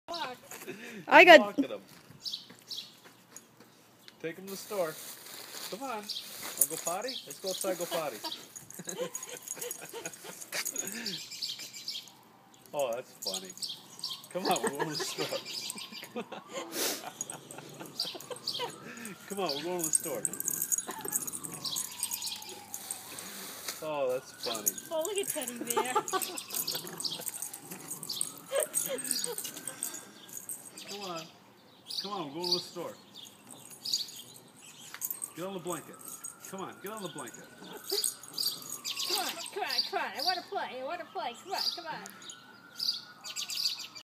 Taking TB for a walk
Jasper the Rat Terrier is trying to take Teddy Bear the Pug for a walk.